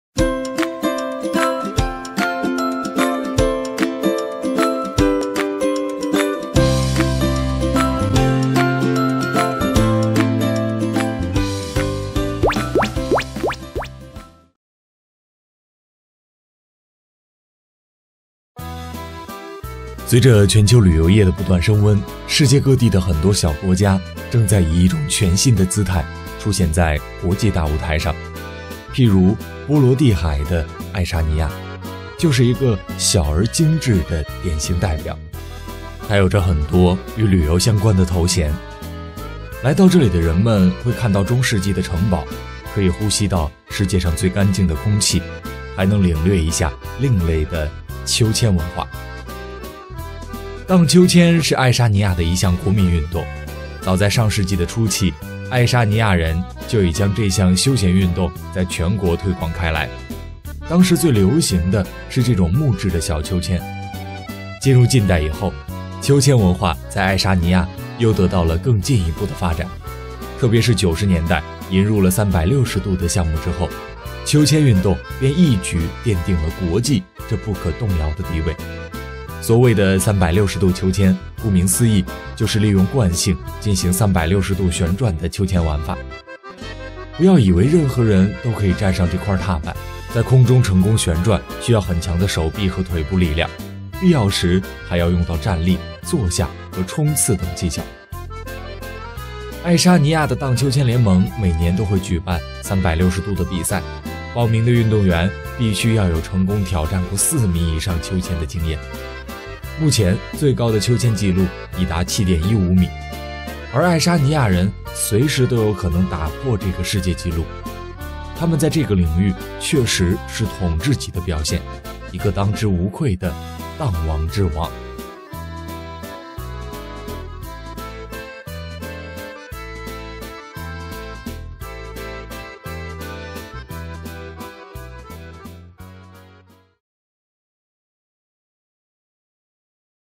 15米的翻滚！爱沙尼亚的秋千一玩就停不下来【寰球大百科233】
随着全球旅游业的不断升温，世界各地的很多小国家正在以一种全新的姿态出现在国际大舞台上。譬如波罗的海的爱沙尼亚，就是一个“小而精致”的典型代表。它有着很多与旅游相关的头衔，来到这里的人们会看到中世纪的城堡，可以呼吸到世界上最干净的空气，还能领略一下另类的“秋千文化”。